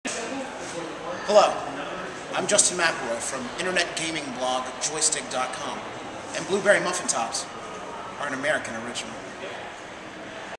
AN AMERICAN ORIGINAL ON AN AMERICAN ORIGINAL - Blueberry Muffin Tops
Justin McElroy of famed gamer blog, Joystiq.com let's us know what Blueberry Muffin Tops is.